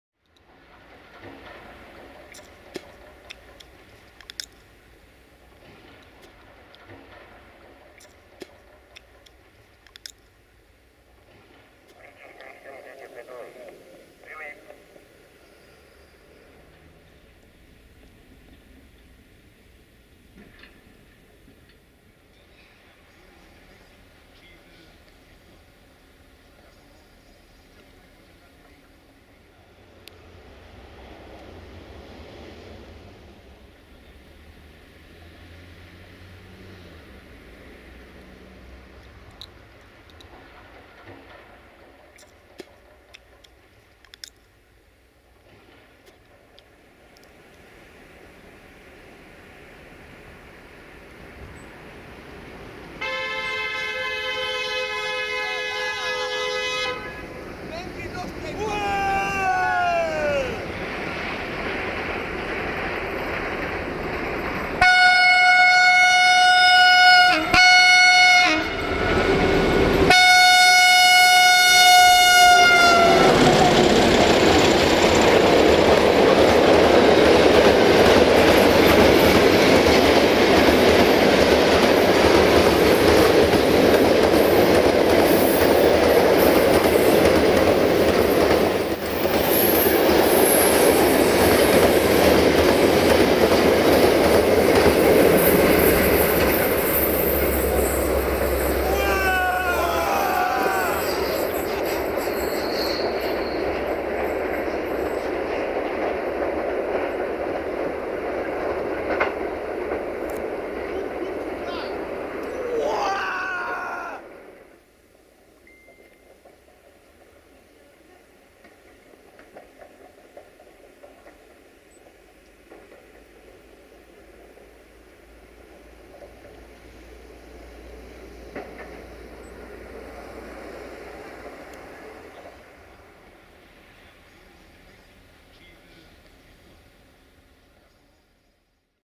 Elodia~DA 62-1404-8 in Actiune/Action & Mecanic Super Enginer in Oradea Est - 10 February 2017
1:CFR MARFA Freight Train/Marfar/Guterzug- 060-DA/LDE2100 Elodia` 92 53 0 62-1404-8 RO-SNTFM

10 February 2017